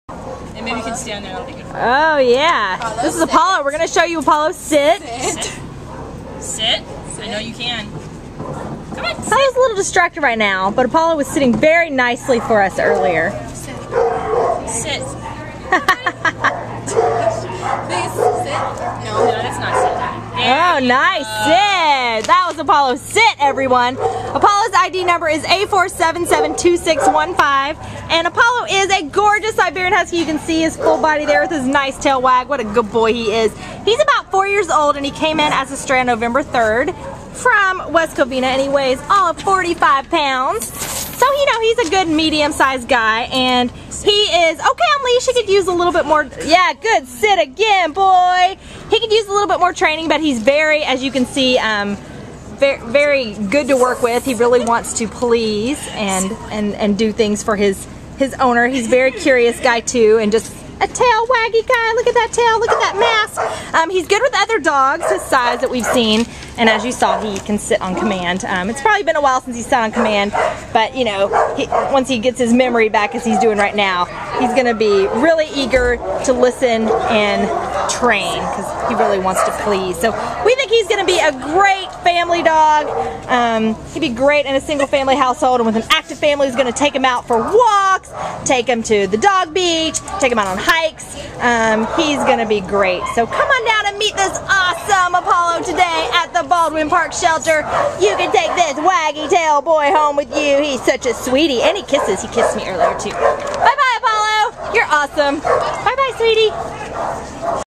A4772615 Apollo
A4772615 Apollo is a magnificent 4-year-old red and white male Siberian Husky who was found in West Covina and brought to the Baldwin Park Animal Care Center on November 3rd. Weighing 45 lbs, Apollo’s name (derived from a sun god) is fitting because of his gorgeous, red coat and strong, powerful frame. But don’t let his regal and sporty look fool you. This boy is also one big sweetheart! A smiley and eager guy whose tail is always on the wag, Apollo will do anything for a treat (which he also happens to take ever-so-gently from your hand). Already able to sit on command and showing signs of being housebroken, he could use some additional training on leash; but we expect him to go to the head of his doggie class since he is so treat-motivated. Because he is a bit excitable around other dogs (at least in the shelter environment), he should be properly introduced to other pups. But it is just this zest that makes him perfect for someone looking for a hiking, running, and general outdoor activity partner. Apollo will make a fun and rewarding indoor pet for any active individual or family with children old enough to play fetch with him. He will be happiest in a single-family home, preferably one with a yard where he can run and play.